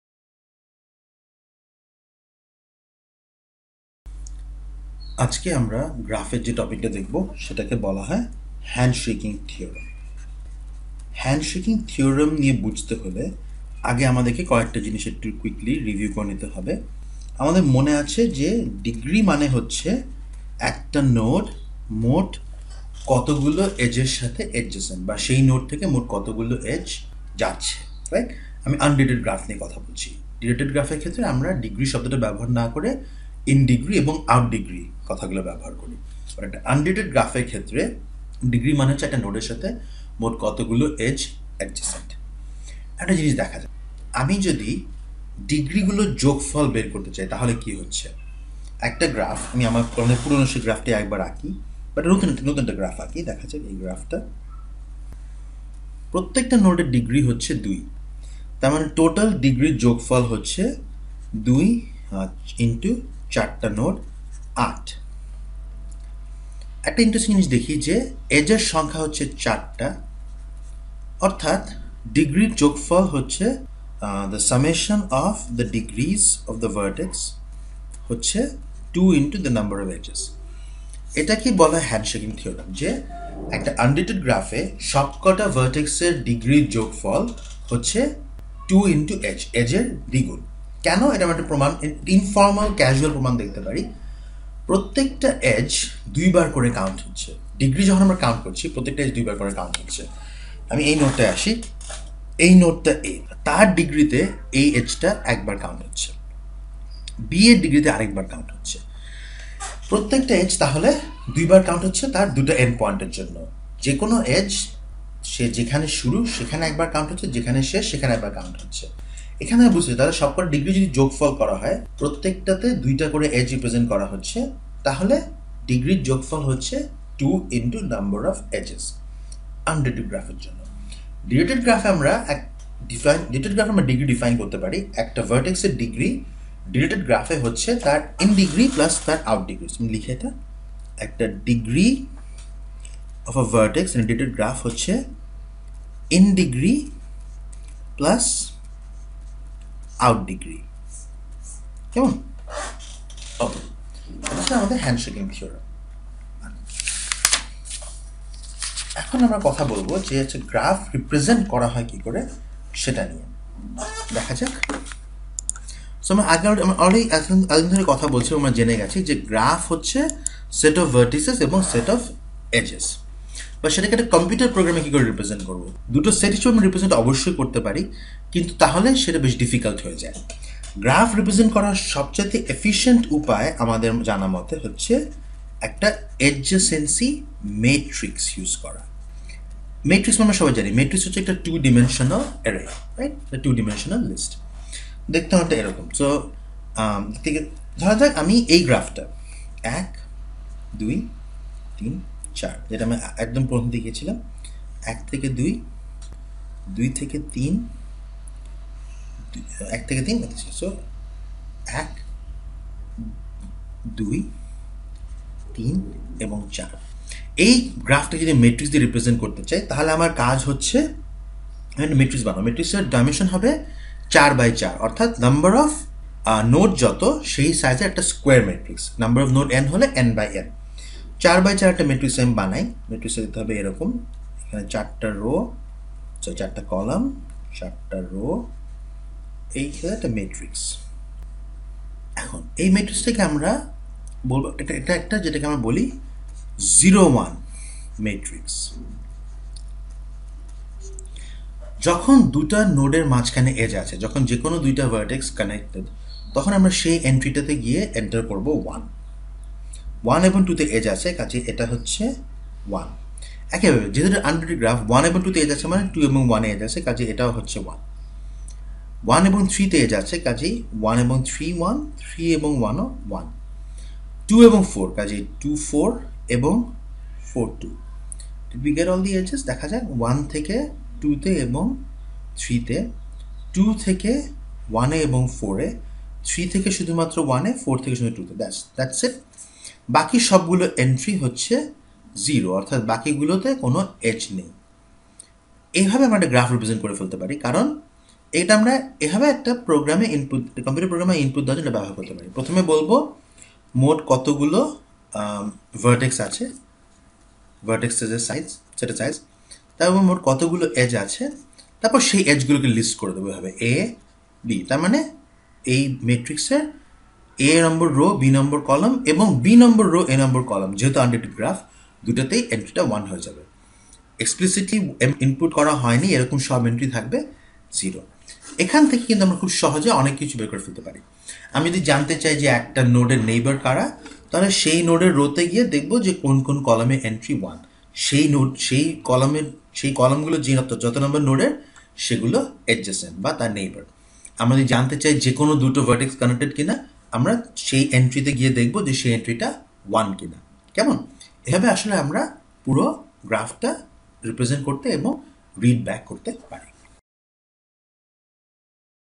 CS101 - Discrete Mathematics - Handshaking Theorem (হ্যান্ডশেকিং থেওরেম)
Discrete Mathematics
Week 08
Lecture 01 - Handshaking Theorem

বিচ্ছিন্ন গণিত
অষ্টম সপ্তাহ
লেকচার ০১ - হ্যান্ডশেকিং থেওরেম